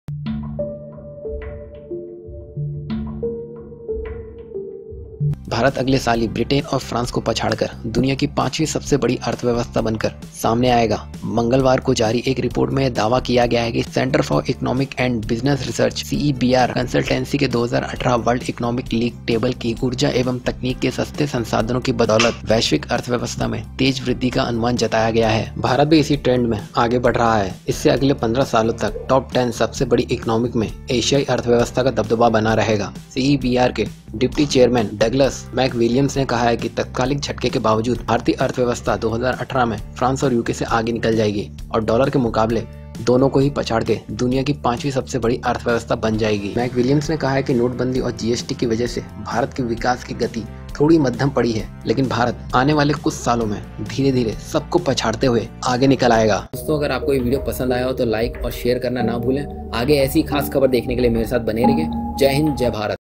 India 2018 में ही UK और France को पछाड़कर पांचवीं सबसे बड़ी \world’s fifth-largest economy India
India will  the United Kingdom and France in 2018 to become the world’s fifth-largest economy in terms of the United States dollar, an economics consultancy based in Britain said on Tuesday. The Centre for Economics and Business Research’s 2018 World Economic League Table also said that Asian economies will dominate the list of the top ten largest economies over the next 15 years,

Sound Source